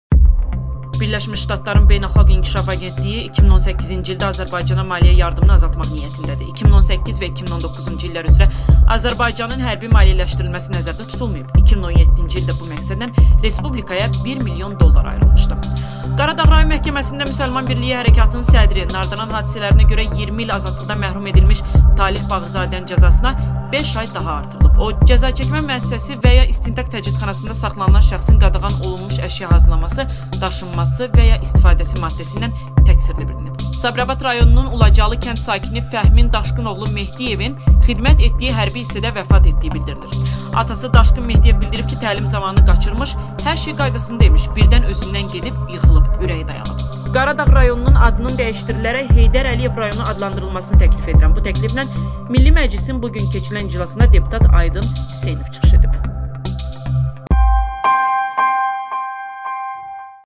1DƏQİQƏ: Artıq ABŞ Azərbaycan hərbisini maliyyələşdirməyəcək
Taleh Bağırzadənin cəzası artırıldı
Hərbi hissədə əsgər vəfat edib
Deputat: Qaradağın adı dəyişdirilib Heydər Əliyev rayonu olsun